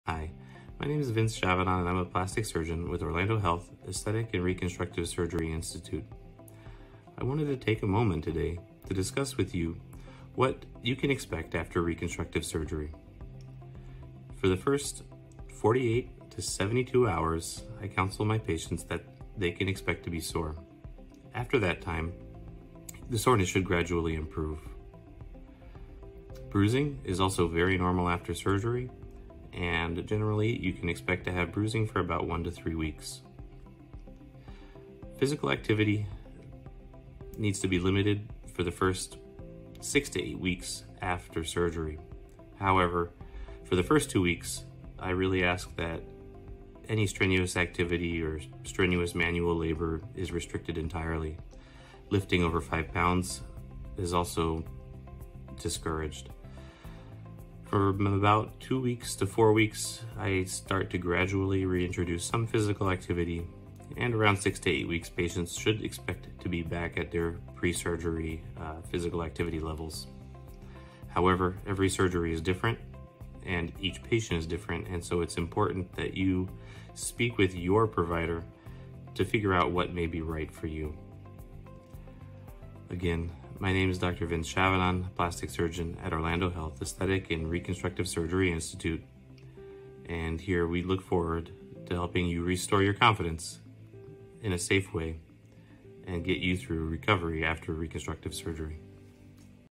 Journey to Recovery After Reconstructive Surgery
Discover the journey to recovery after reconstructive surgery with Dr. Vincent Chavanon, a board-certified plastic and reconstructive surgeon with the Orlando Health Aesthetic and Reconstructive Surgery Institute. At Orlando Health, our dedicated team is committed to helping you regain your confidence in a safe and supportive environment.